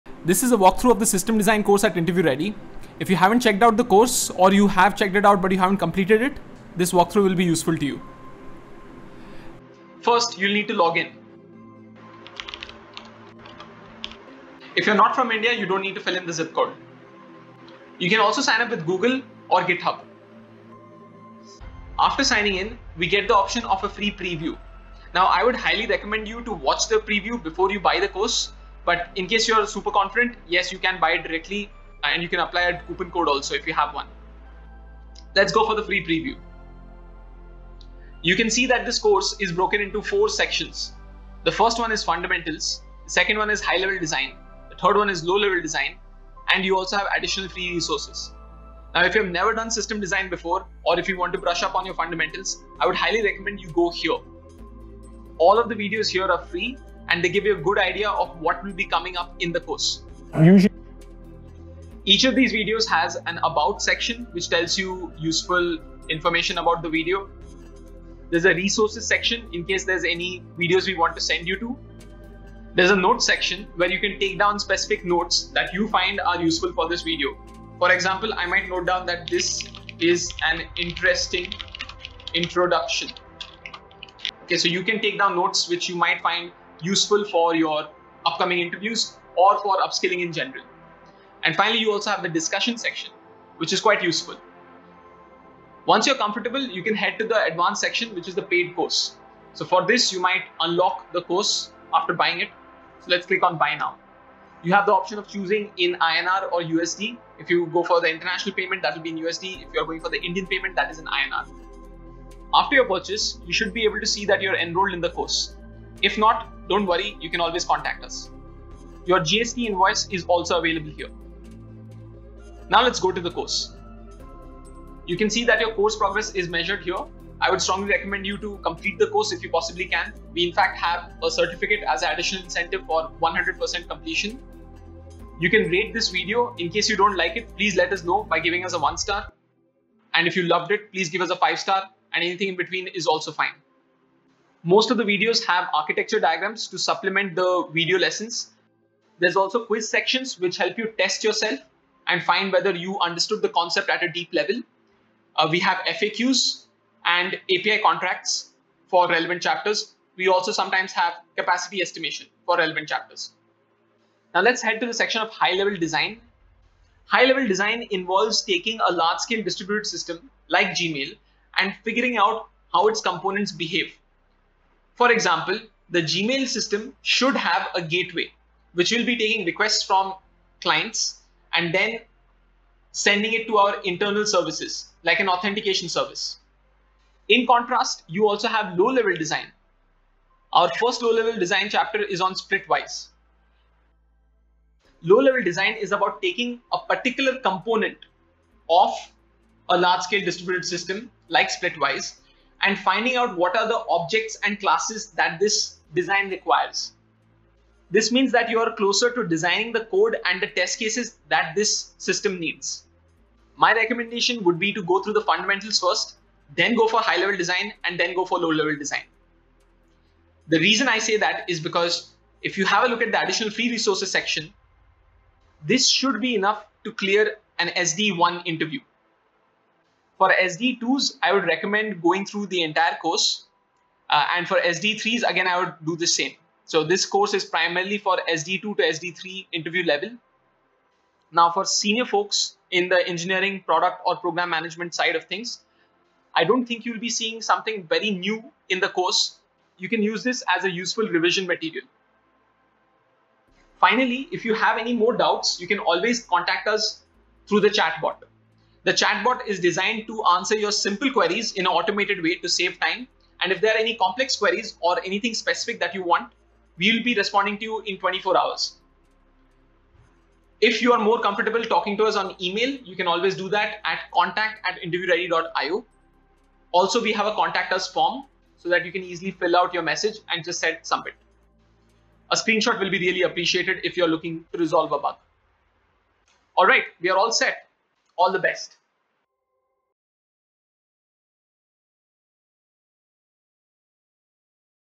System Design Walkthrough at InterviewReady - Designed for SDE 1 to SDE 3 Interview Preparation
This is a detailed walkthrough of the system design course at InterviewReady. If you are looking to prepare for interviews, this walkthrough will help.

The walkthrough describes how high-level, low level and fundamental concepts of design are separated into sections inside the course.

The platform includes note-taking, quizzes, architecture diagrams, and discussion forums. We also have monthly live zoom classes to discuss doubts and chat about design concepts!